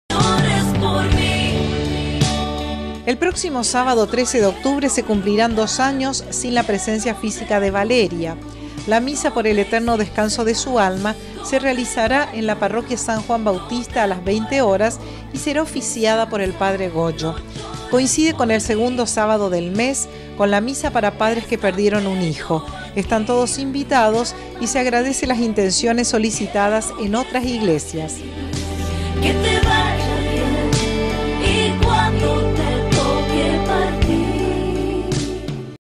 Misa Rossana Valeria Romero Azar
Se cumplen 2 años de la desaparición física de Vale . . . con ese motivo se oficiará una misa, el sábado 13 de octubre a las 20:00 hs en la Parroquia San Juan Bautista . . . . .